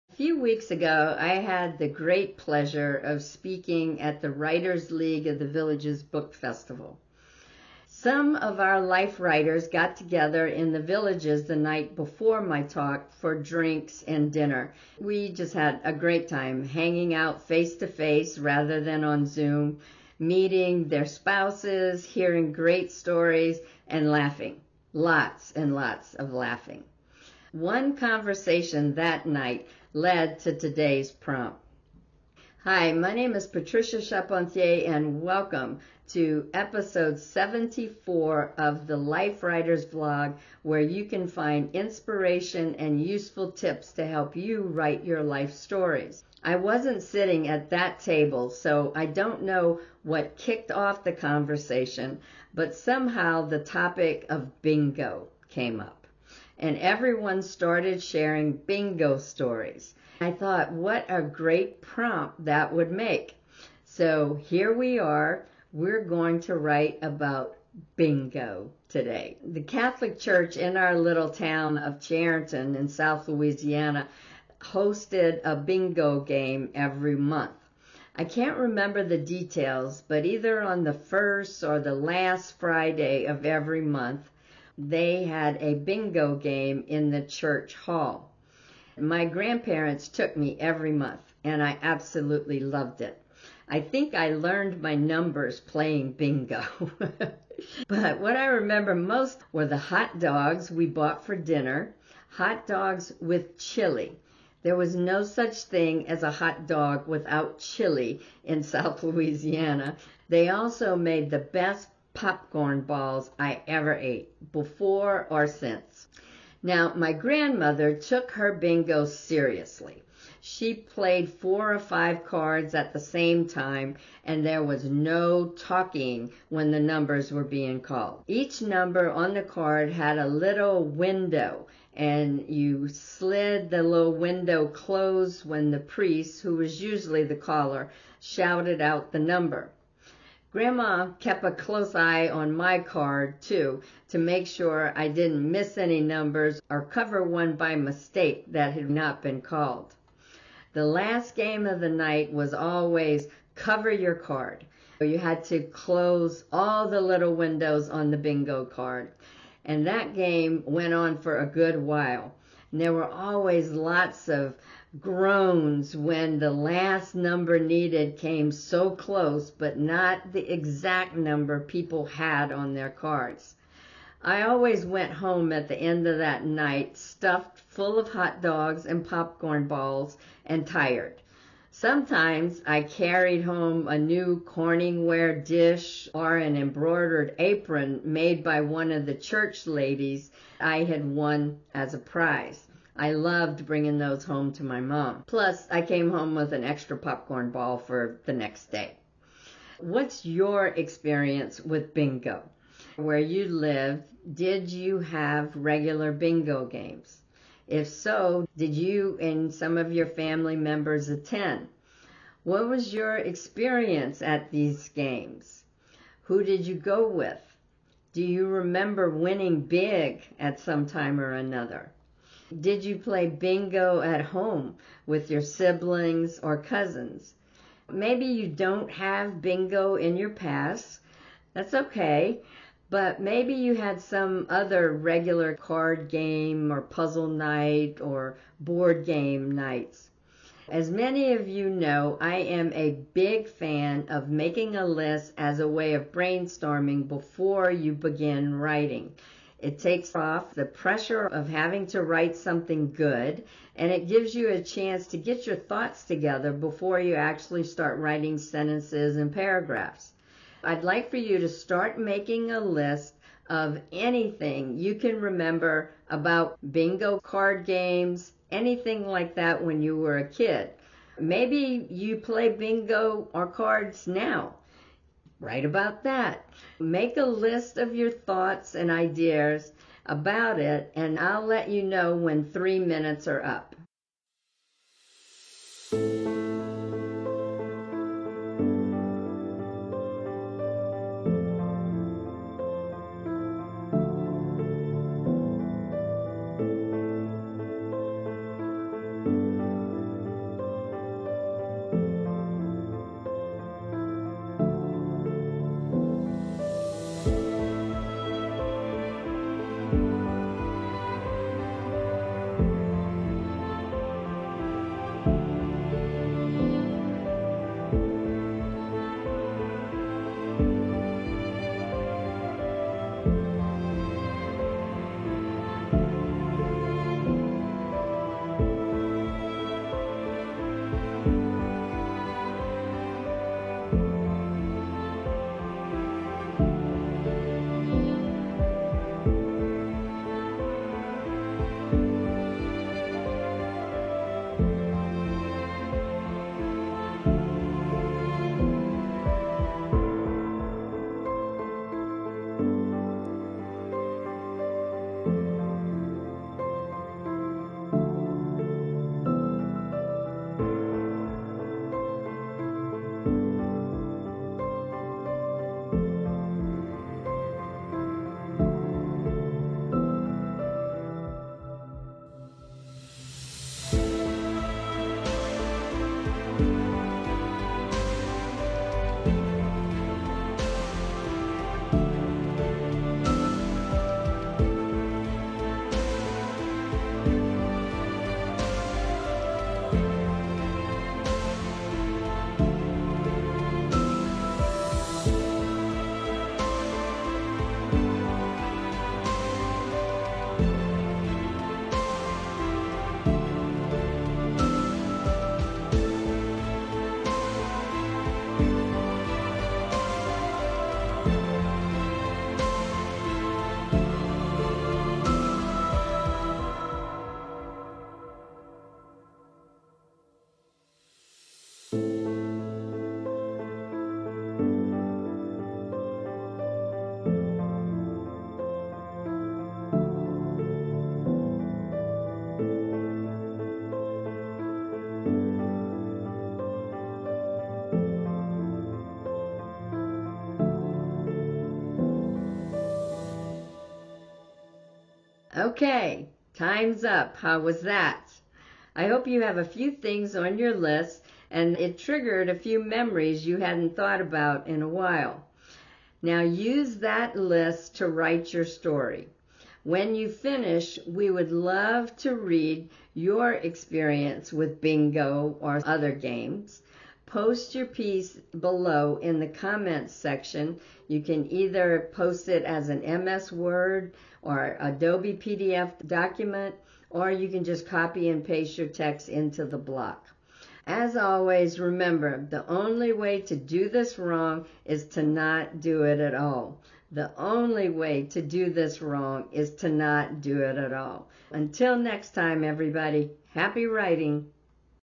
LW Vlog #74 B-I-N-G-O
At a recent face-to-face gathering of our Life Writers, the topic of bingo came up, and I thought what a great writing prompt that would be.

I vividly remember the bingo nights at the Catholic Church hall in my hometown of Charenton, Louisiana. My grandparents took me with them every month, and I think I learned my numbers by playing bingo.

I can still remember the crunchy popcorn balls and the yummy hotdogs--with chili always--the church ladies made and sold.

Let's do a little brainstorming about bingo, and then, we'll write.

If bingo wasn’t a part of your childhood, what similar nights did you enjoy–cards, board games, or puzzles?

Share your bingo memories or other game nights in the comments section below! We’d love to compare notes with you.

But always remember, The only way to do this wrong is to not do it at all!